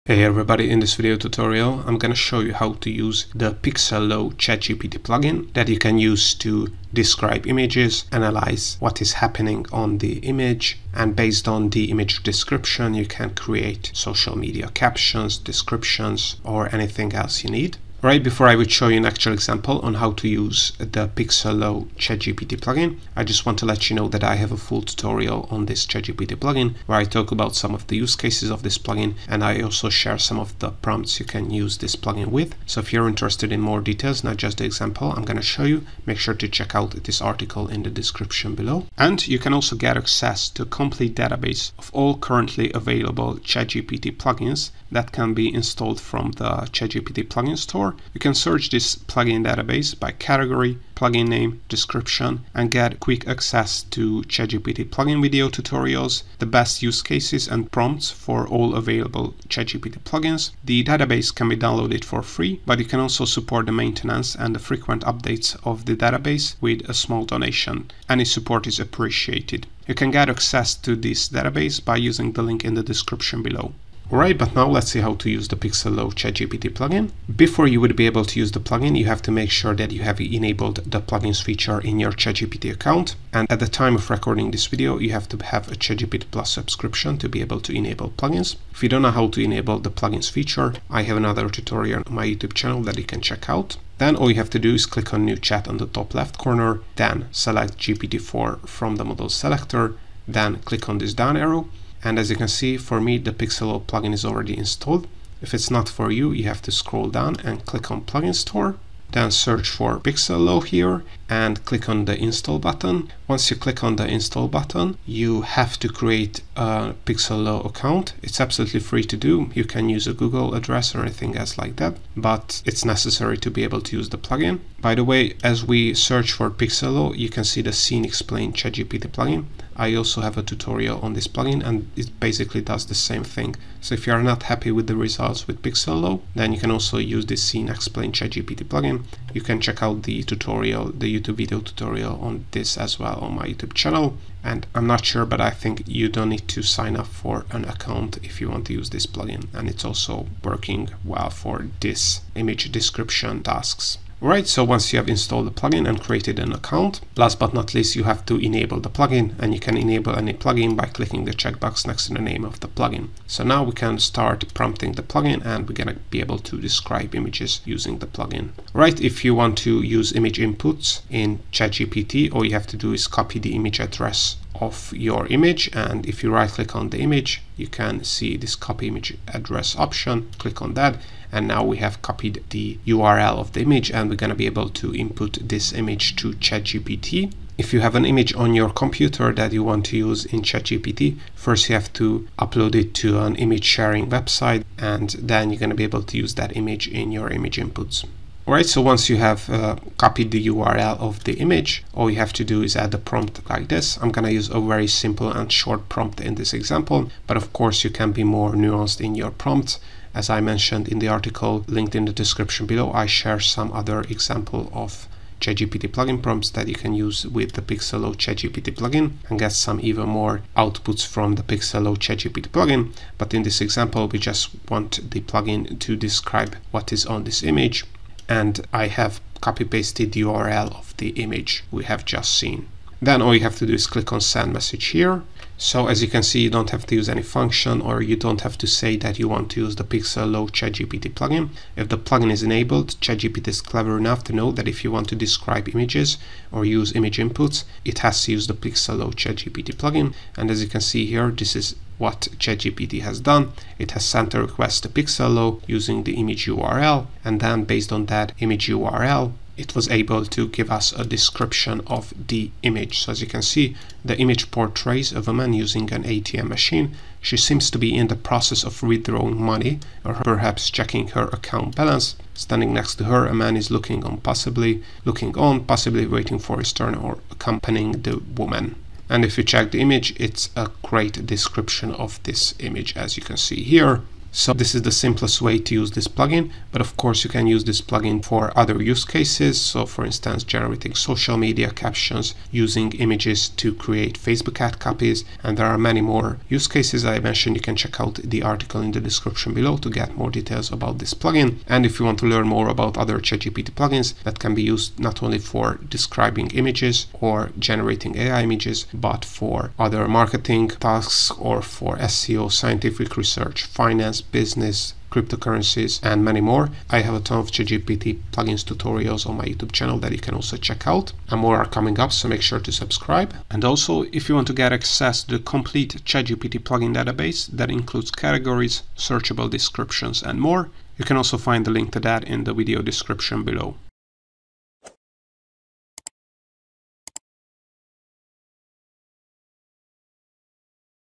How To Use Pixellow ChatGPT Plugin?
. - Learn how to Use the Pixellow ChatGPT Plugin to describe images inside ChatGPT.